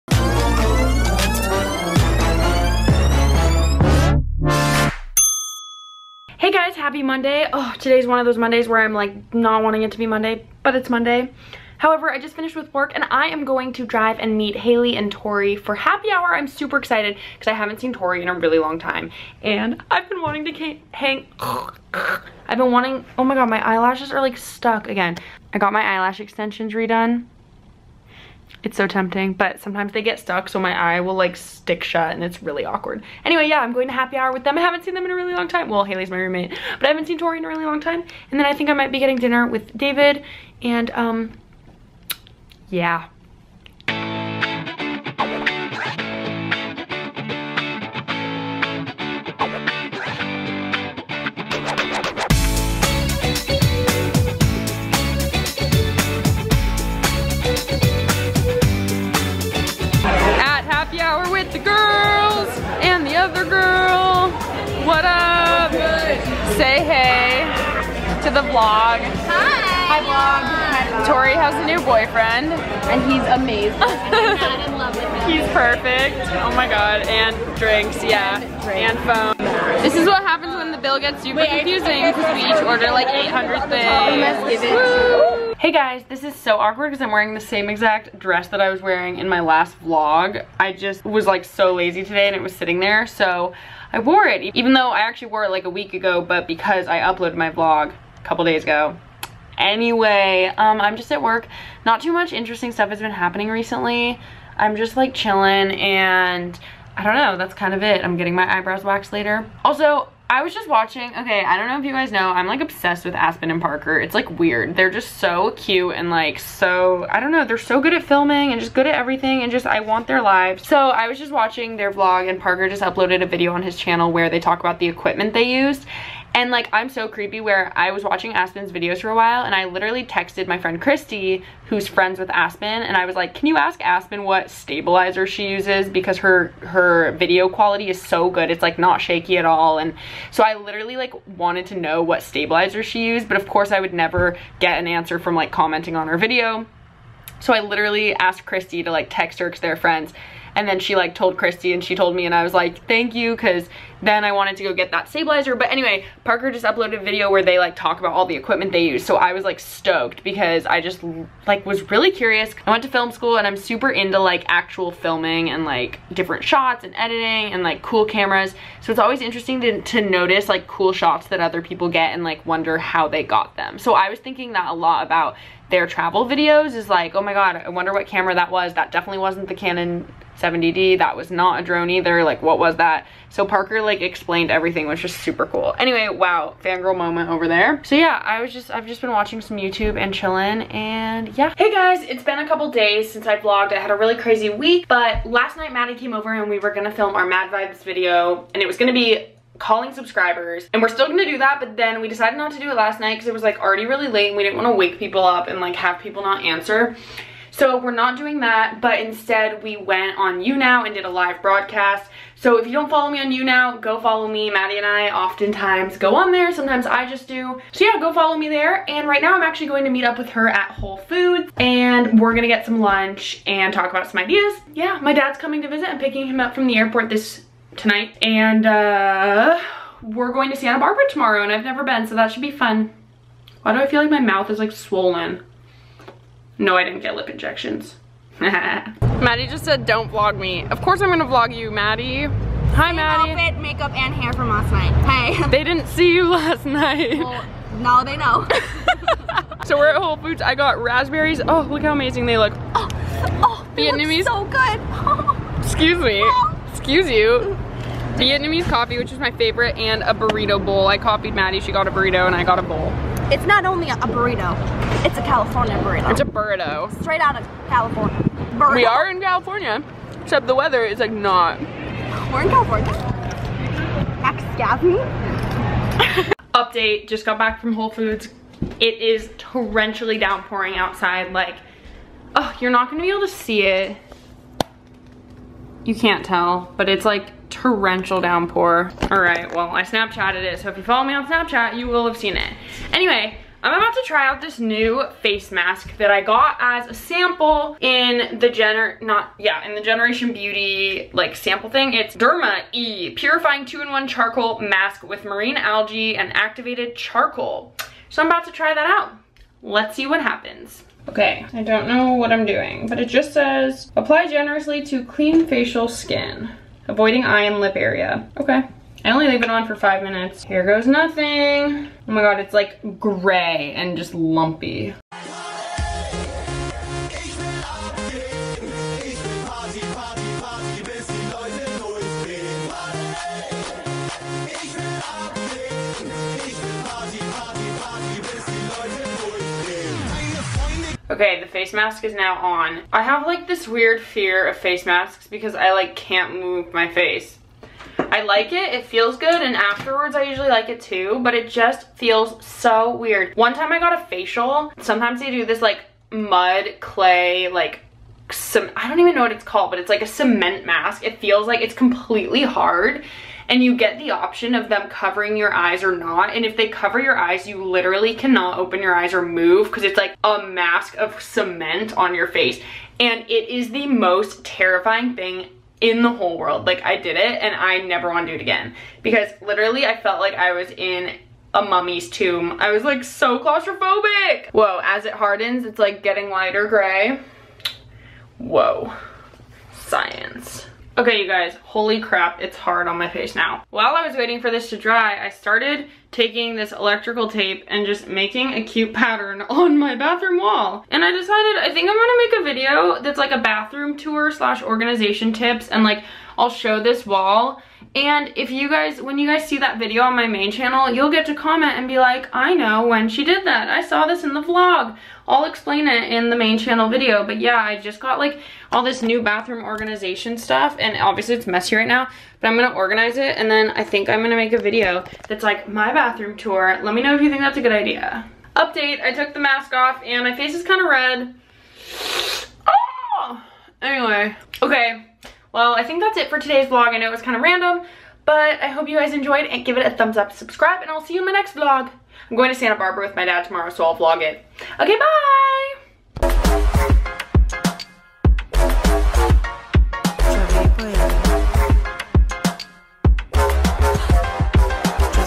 Face Mask Fail! | AdrienneVLOGS
It seriously burned so much... I don't know if face masks are my thing!
→ LET'S BE FRIENDS! ←
✖ Vine - @adrienne_wf

Hi guys! In today's vlog, I get drinks with my friends who I haven't seen in a little while, then I hang with Maddy and try a new kind of face mask! Do you guys like face masks? I can't decide! I also stalk Aspyn and Parker's channels because I'm obsessed with them. lol. Say hi in the comments, I love hearing from you!